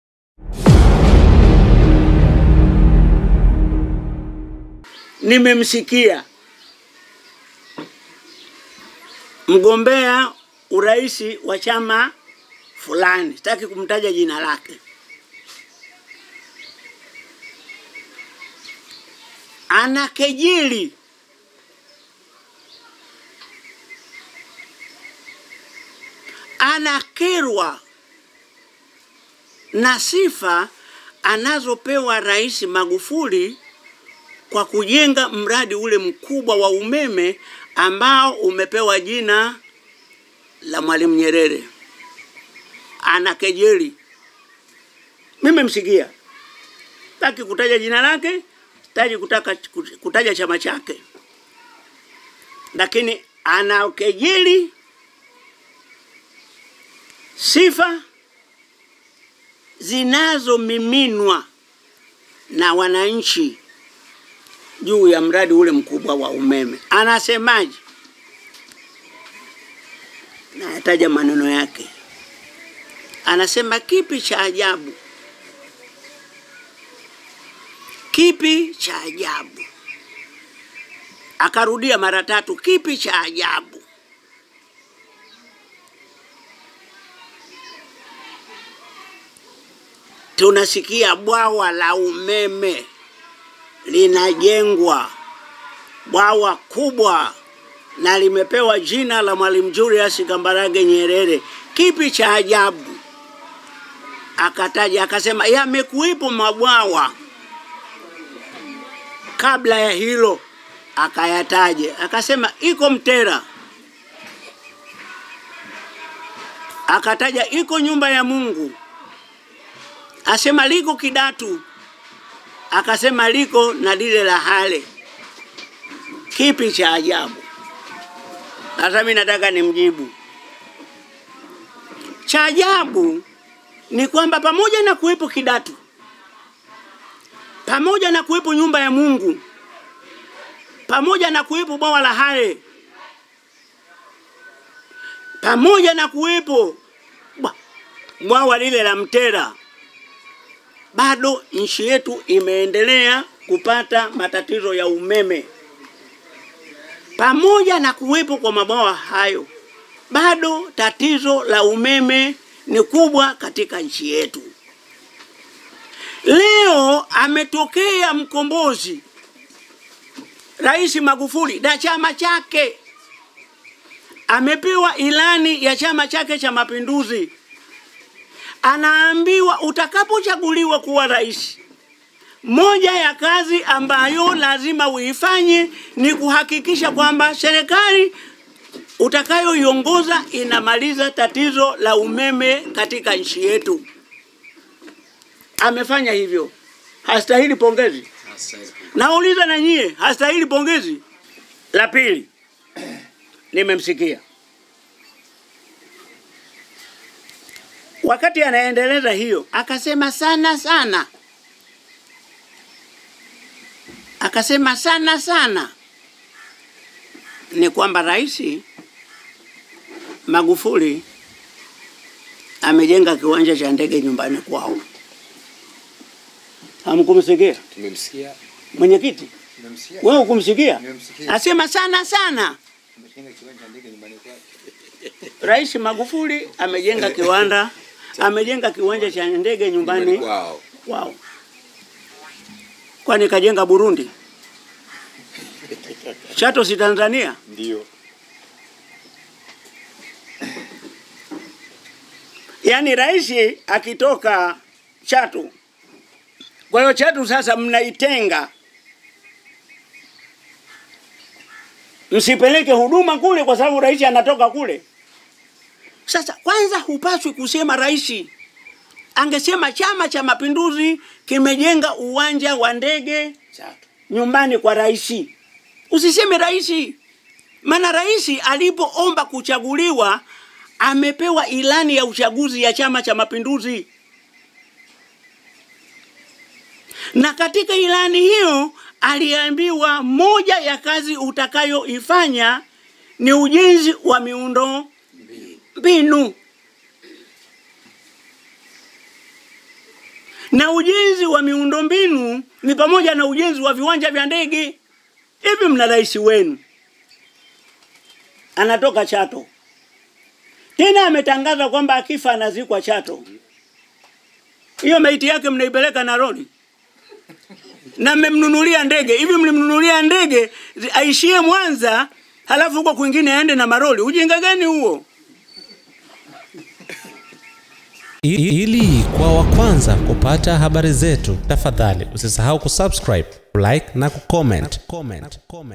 MZEE MAKAMBA AMKINGIA KIFUA JPM "MMEMNUNULIA NDEGE MNATAKA MKAMZIKE NA ROLI?"
Kada wa Chama Cha Mapinduzi, Yusuph Makamba ambaye aliwahi kuwa Mkuu wa MKoa wa Dar es Salaam amemkingia kifua mgombea wa urais kwa tiketi ya chama hicho, Dk. John Magufuli dhidi ya wale wanaombeza kuwa amejenga uwanja wa ndege nyumbani kwao.